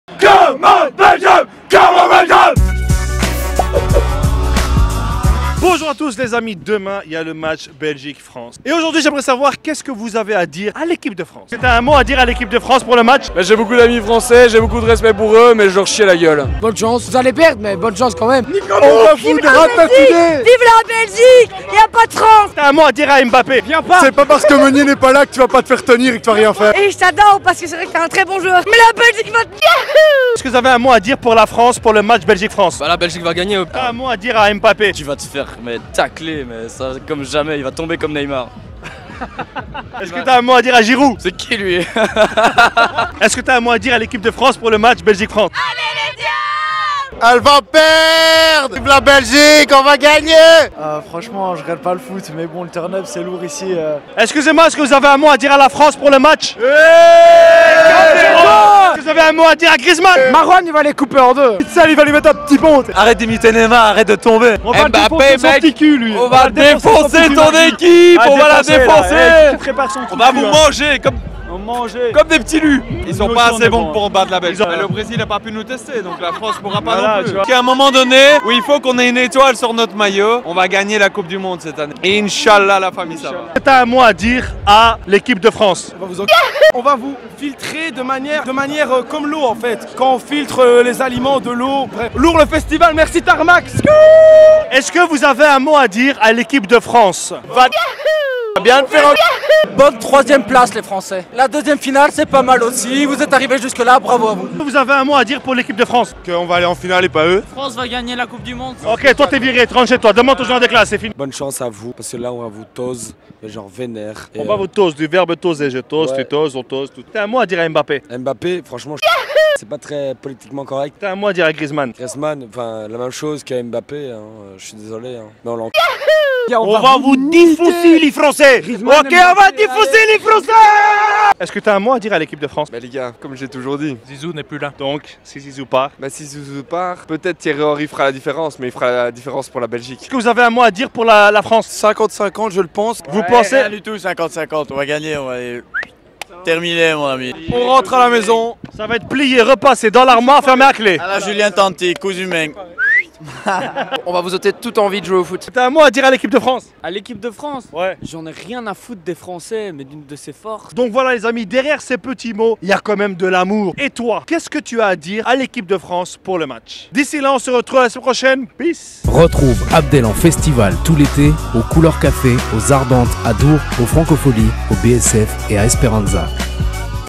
Supporter des diables, quel est ton message à l'équipe de France ?
A la veille de la demi finale opposant la Belgique à la France, Abdel te demande quel est ton message à ton voisin, mets-le en commentaire